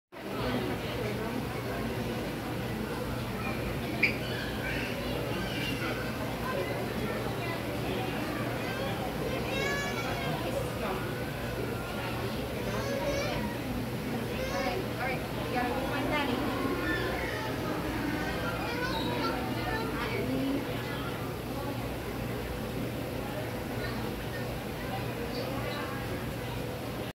Firehead Tetras
Many firehead tetras swimming at New England Aquarium in Boston, Massachusetts.